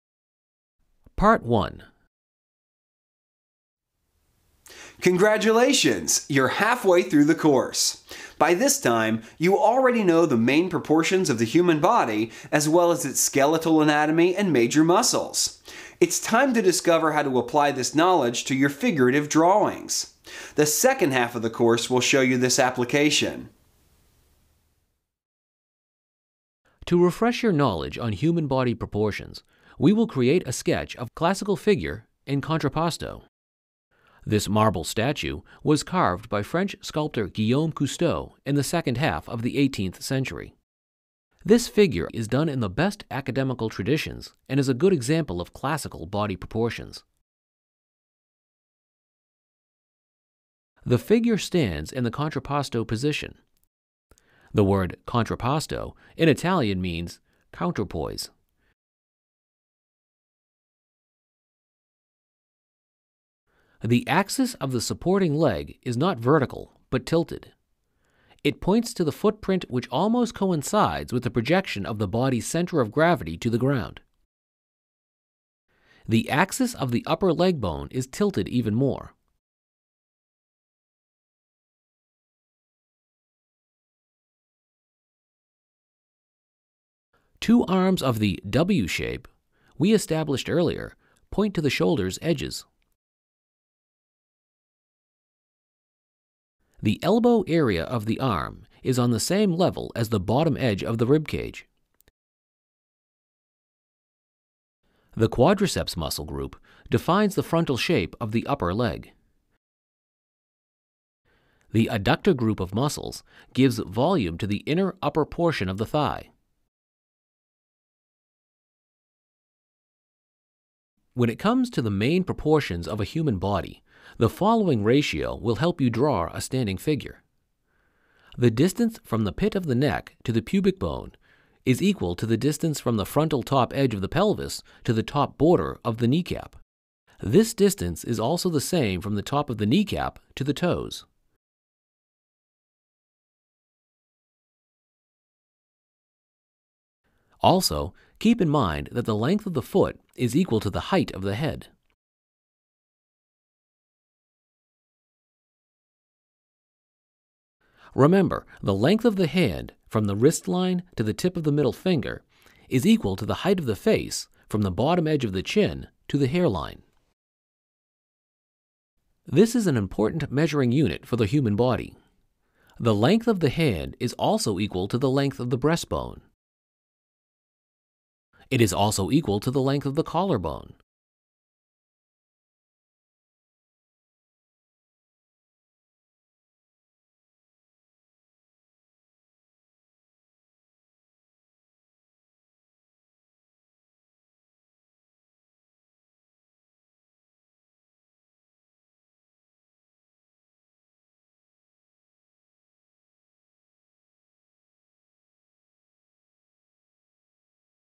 Anatomy of the Human Body - Anatomy Master Class
- Discover Anatomy of the Human Body - Anatomy Video Lesson for Figurative Artists